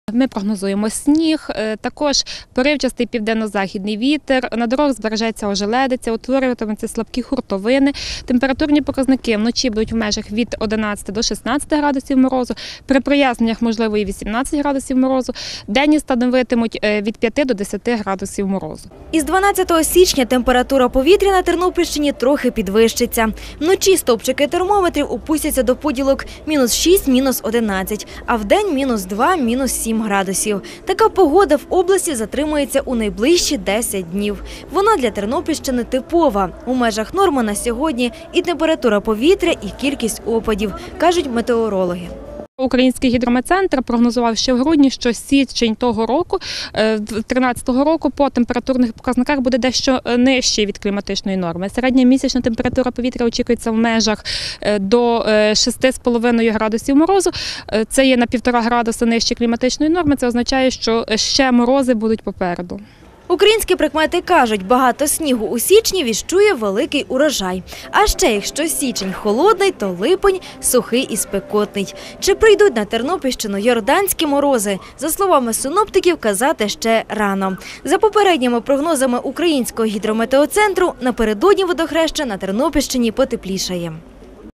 На Тернопільщині потепліє з 12 січня
Тернопільщина у найближчі дні буде під впливом атмферних фронтів. То ж синоптики надалі обіцяють сніг, поривчастий вітер і зниження температури повітря, але ненадовго.